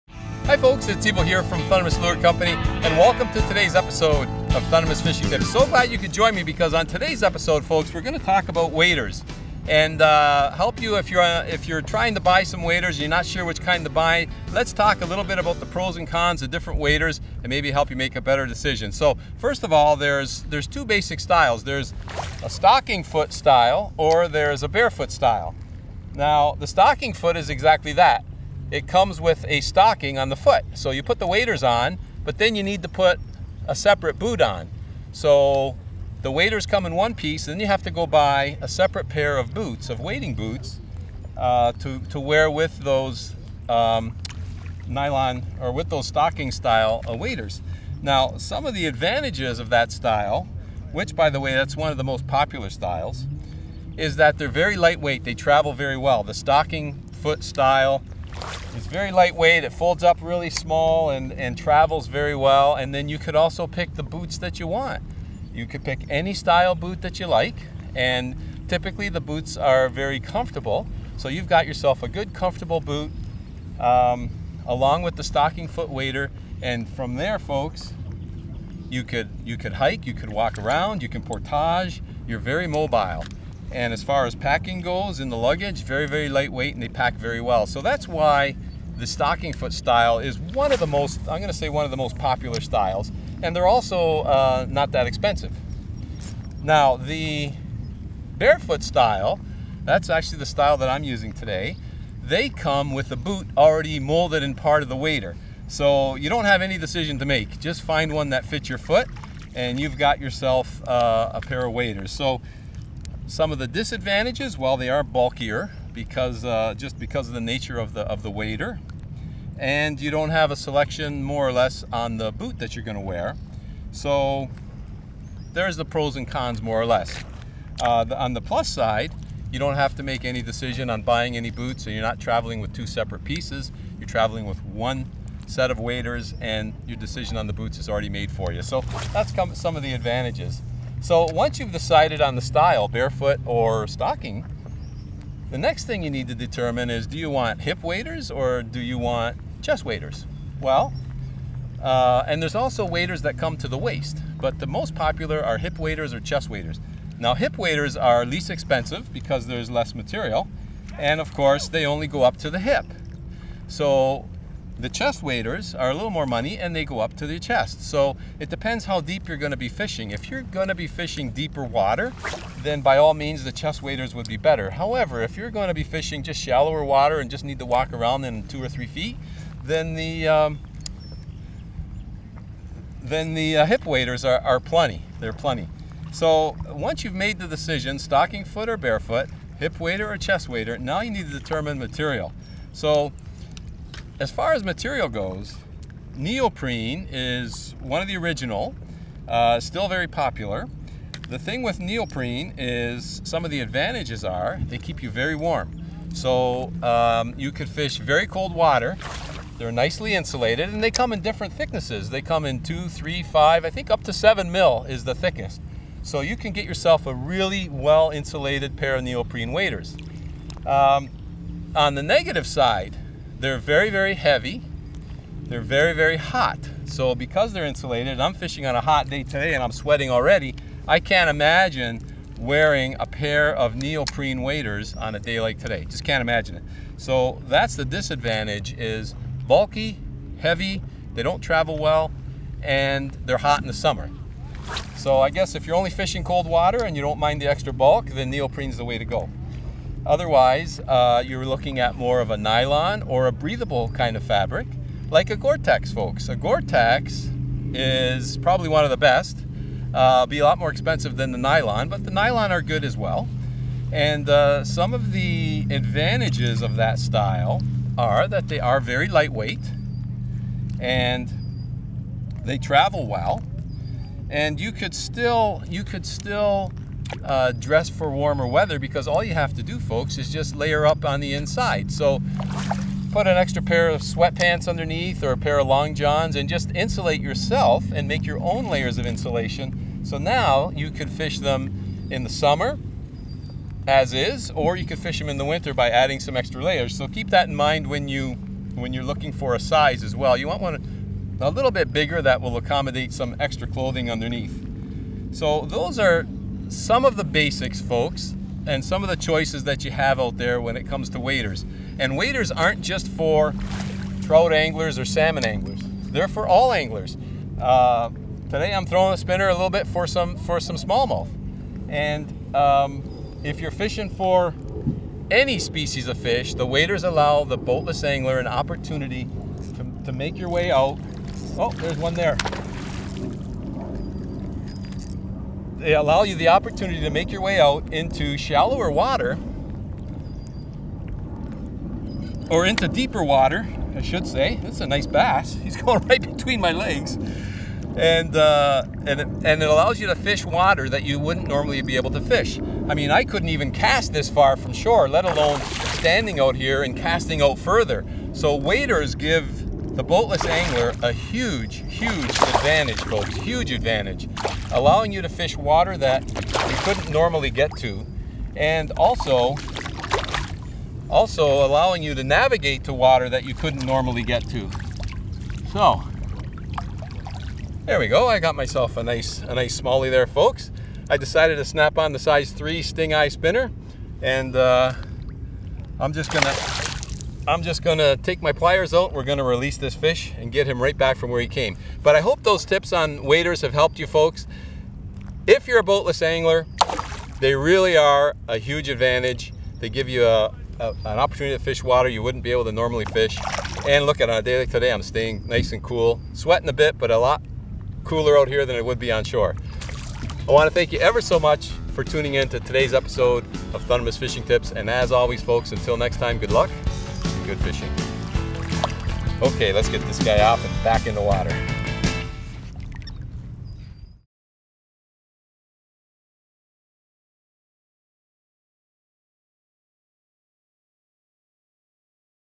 Understanding Chest/Hip Waders - Differences and Tips
Today I want to talk about chest/hip waders. Helping you understand the pros and cons of the different types of hip waders, and what kind of hip waders you might want to buy.